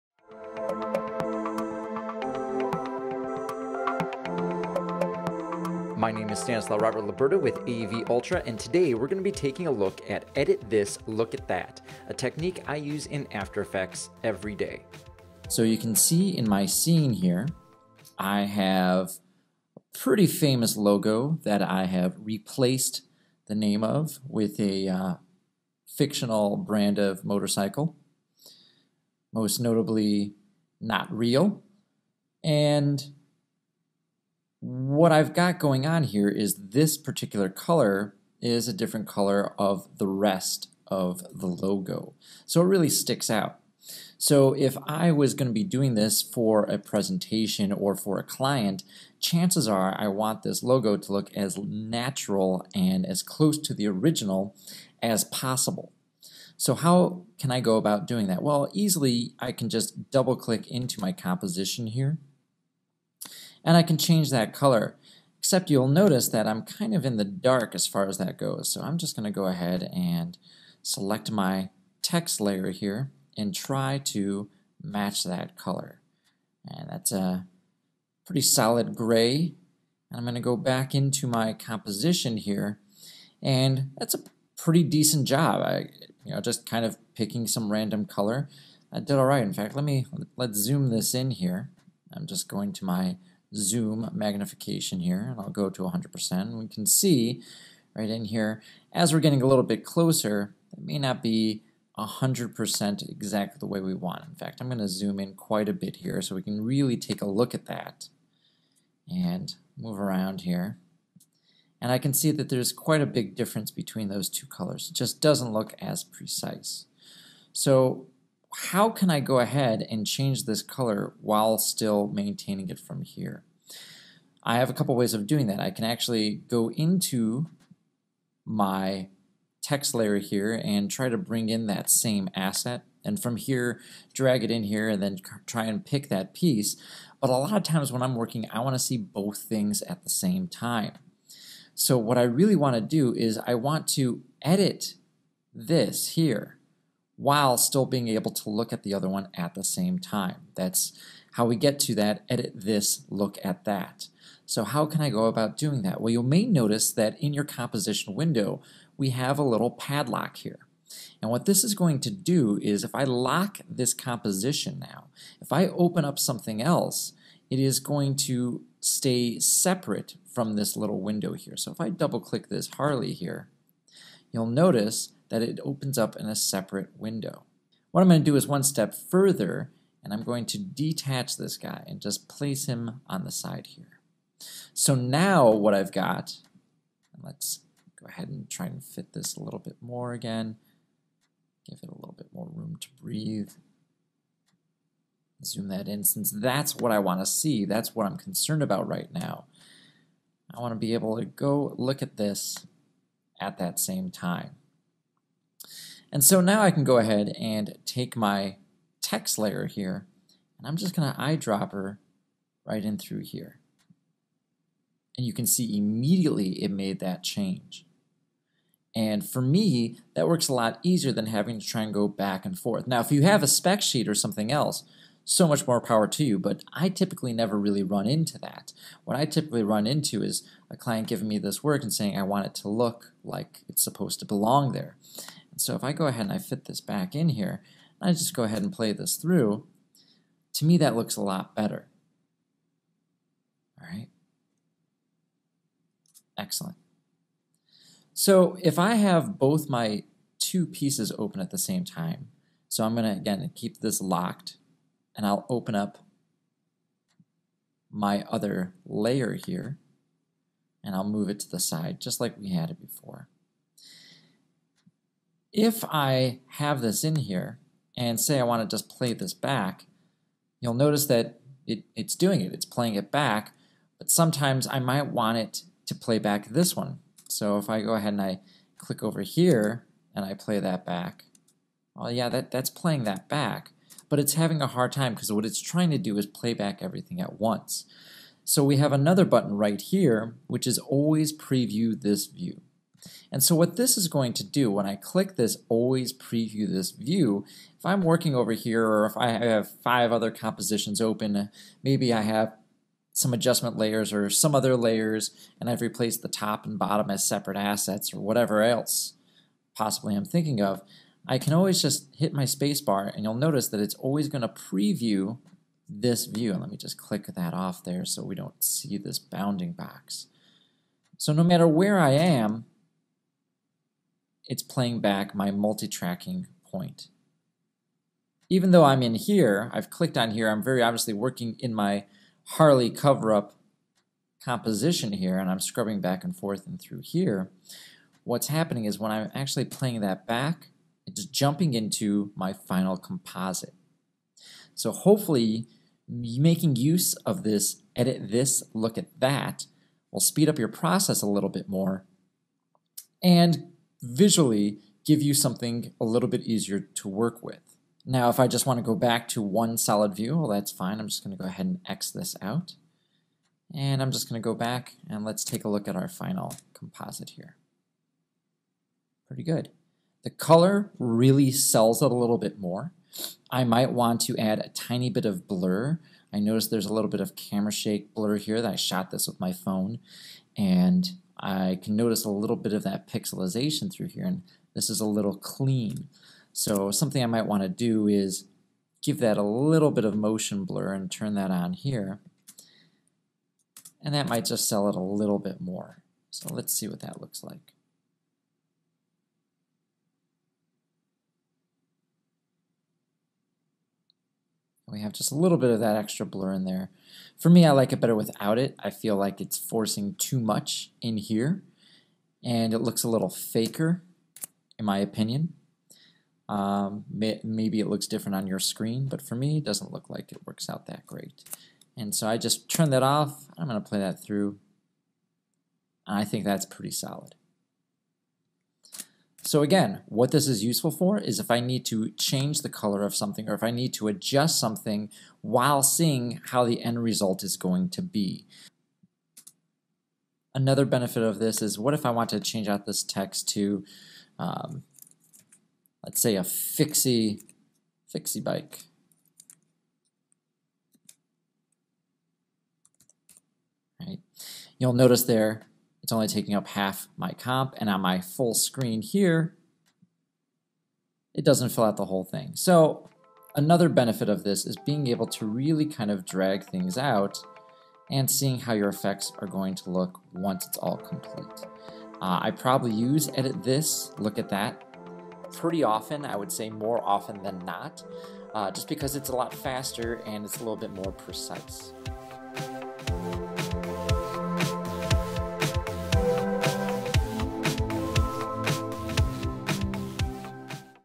AFTER EFFECTS PROFESSIONAL TRAINING lesson 12- Edit this, Look at that
Stanislaw Robert Luberda
Adobe Certified Instructor

Edit this, look at that- or ETLAT for short.

Learn the process of procedural compositions in after effects. Using this guide you will learn how to edit one asset while viewing the end result in real time.

Topics covered in this lesson:
0:15 What is ETLAT?
2:40 Locking Compositions
3:10 Separating Composition Windows
4:50 Always preview this view
7:25 Making changes in real time